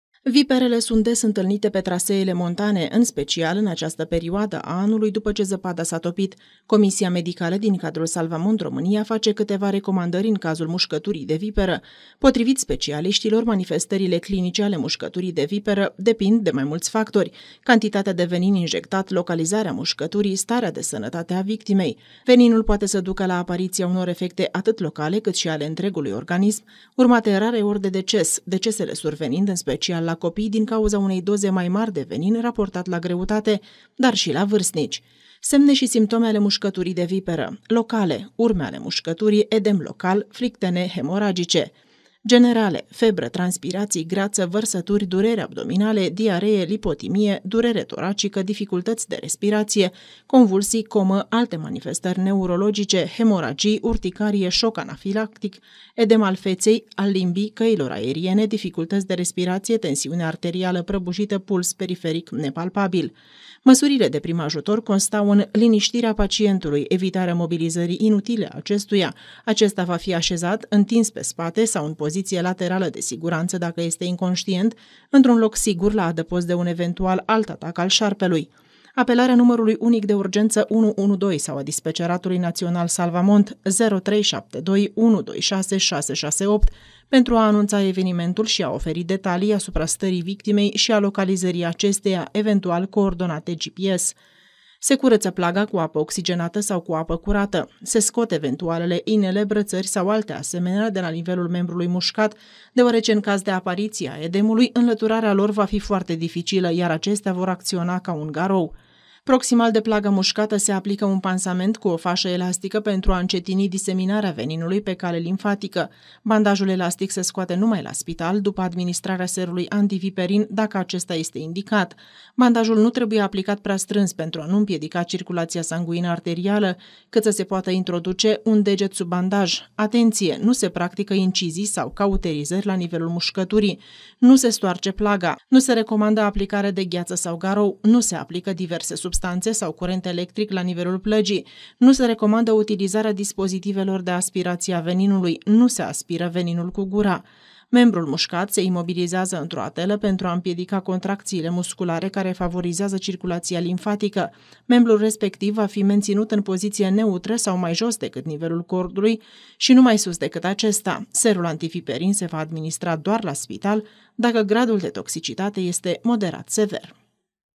Jurnal MUSCEL TV 12.05.2022 Ce trebuie să faceți dacă sunteți mușcați de viperă
Cum în zona muntoasă zăpada a început să se topească au început să fie văzute viperele. Specialiștii vă fac câteva recomandări în cazul în care sunteți mușcați de un asemenea șarpe.